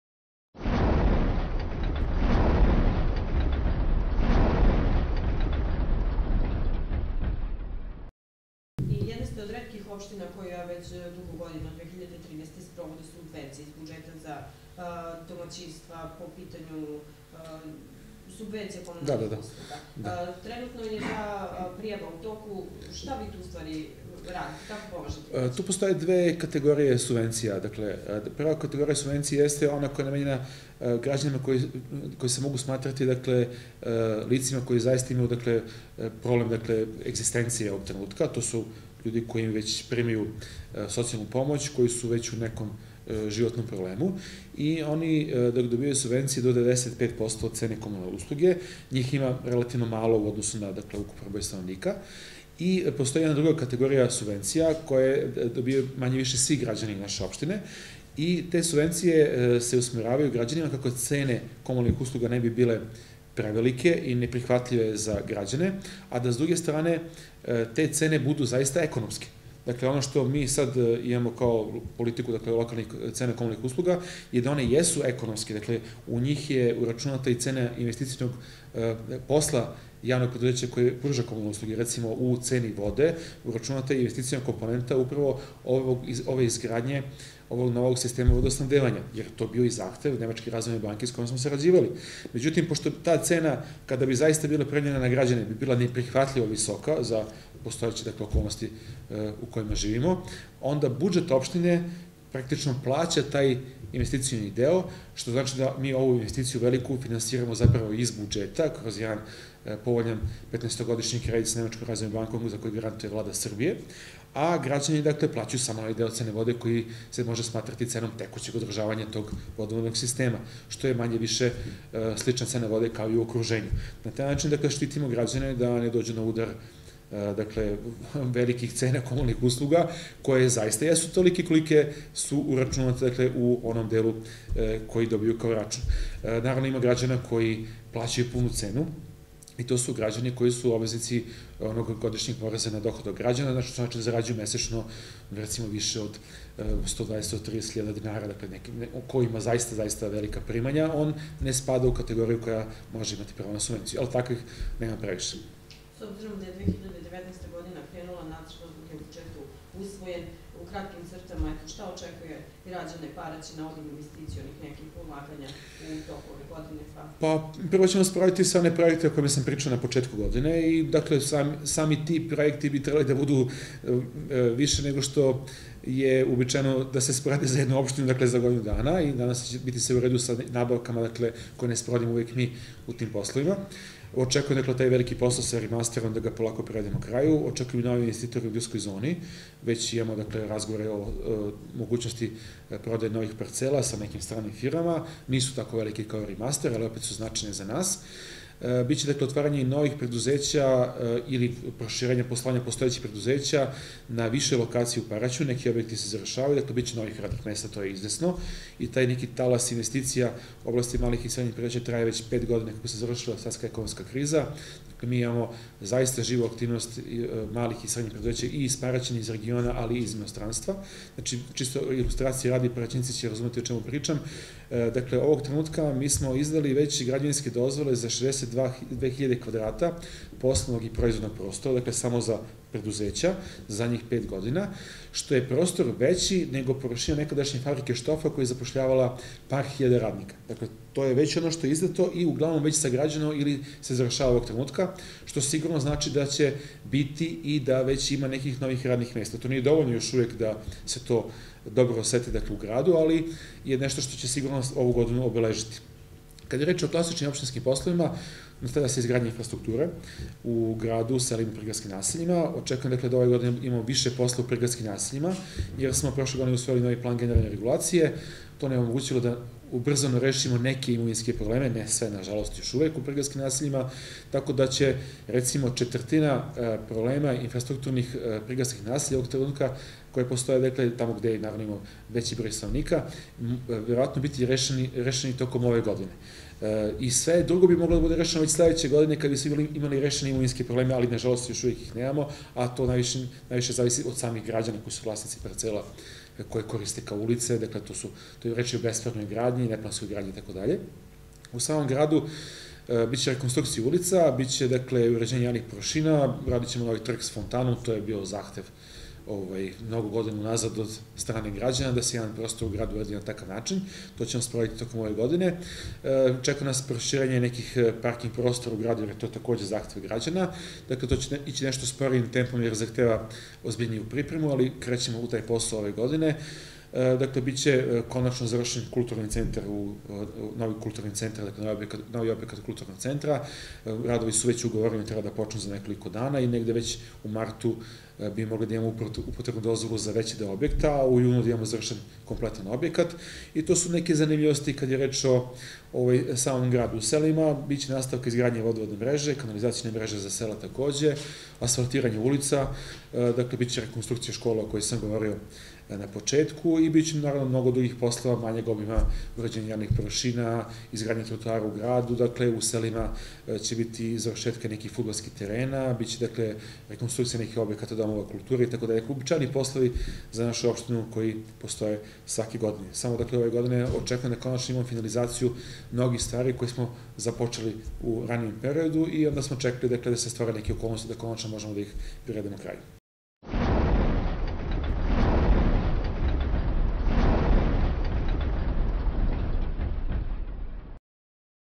Saša Paunović IV deo - O subvencijama za komunalne usluge i investicije u 2019.
Intervju sa predsednikom opštine Paraćin Sašom Paunovićem.

Intervju je podeljen u šest delova, koje možete pogledati na našem youtube kanalu.